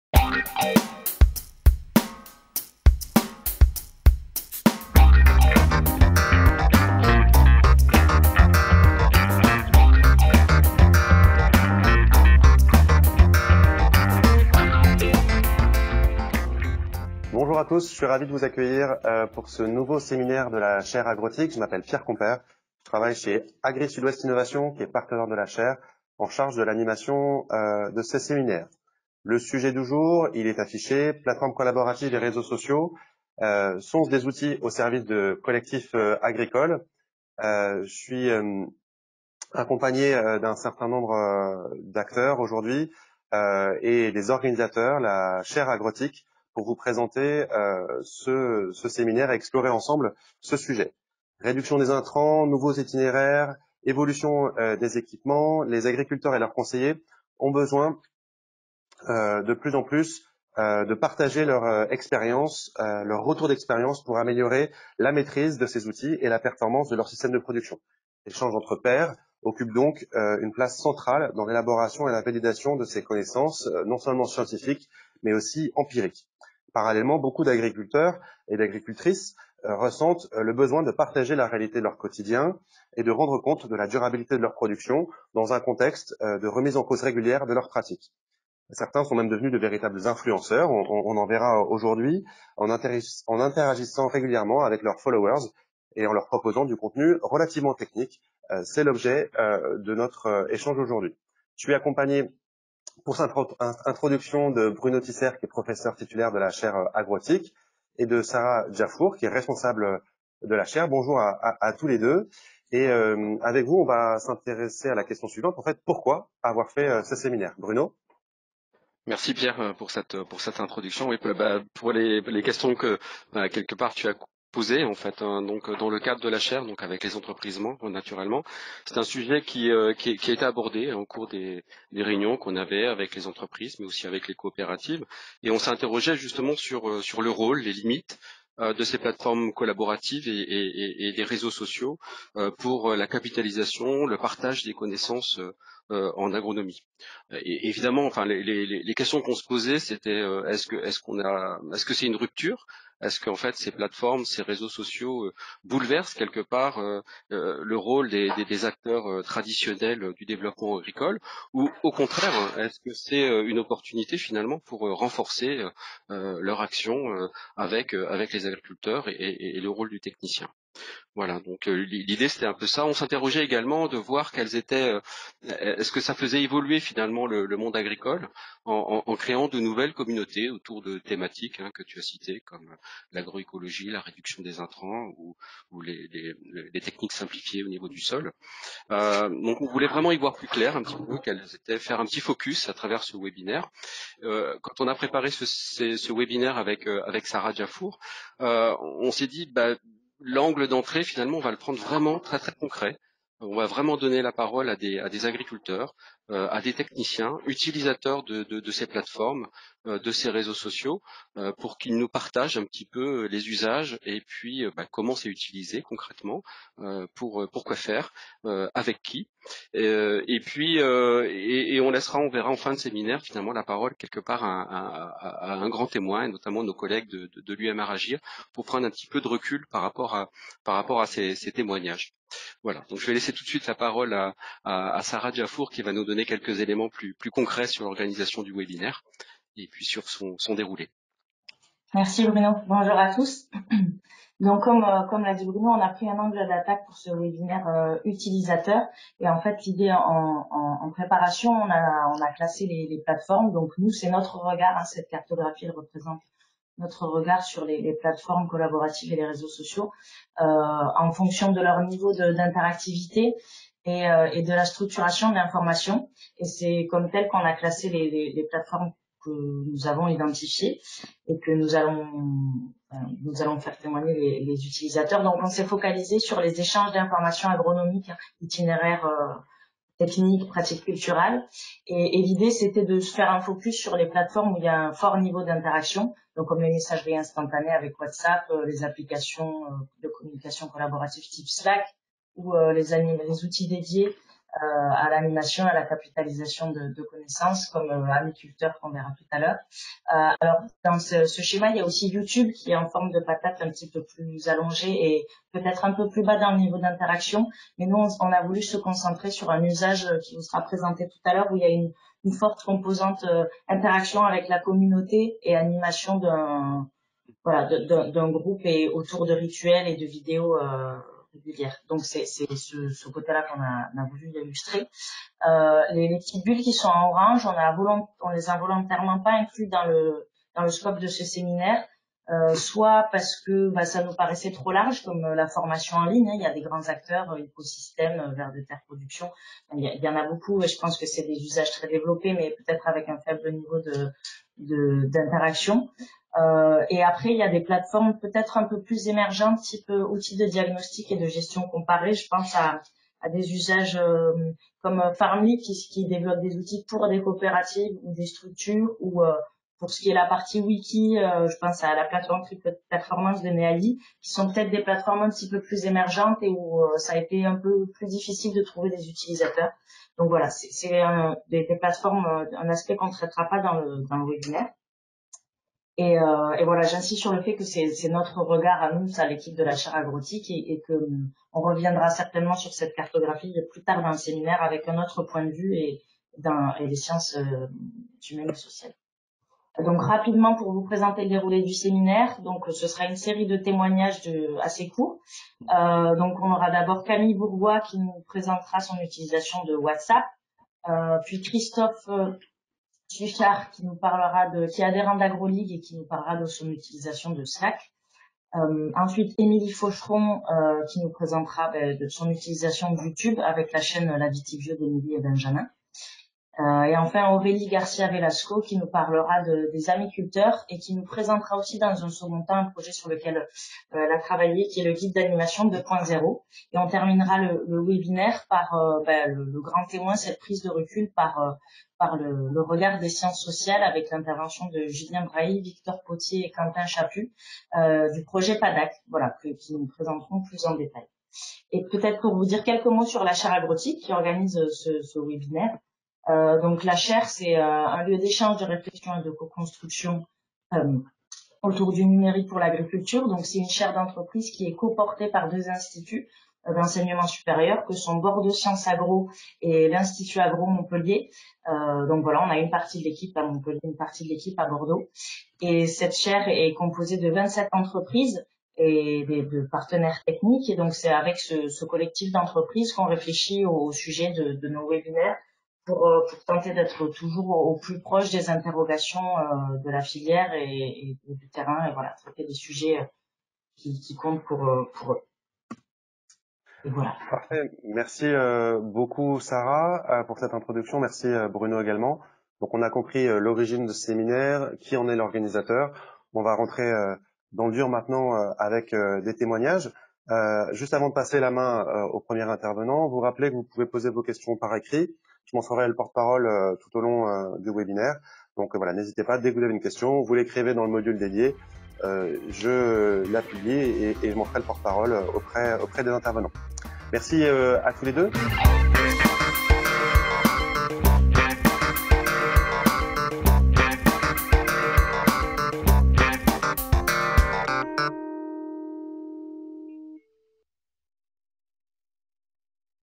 Introduction - Plateformes collaboratives et réseaux sociaux
Ce webinaire organisé par la chaire AgroTIC s'est déroulé le 26 avril 2022.

"Réduction des intrants, nouveaux itinéraires, évolution des équipements : les agriculteurs et leurs conseillers ont besoin de nouvelles références techniques, davantage contextualisées, et de partager leurs retours d’expériences pour améliorer la maîtrise de ces outils et la performance de leurs systèmes de production.

L’échange entre pairs occupe alors une place centrale dans l’élaboration et la validation de ces connaissances scientifiques et empiriques. Parallèlement, beaucoup d’agriculteurs ressentent le besoin de partager la réalité de leur quotidien et de rendre compte de la durabilité de leurs productions dans un contexte de remise en cause régulière de leurs pratiques. Certains sont même devenus de véritables influenceurs en interagissant régulièrement avec leurs followers et en leurs proposant du contenu.

Bousculent-ils ou renforcent-ils les acteurs traditionnels du développement agricole ? Permettent-ils la création et l’animation de nouvelles communautés ?"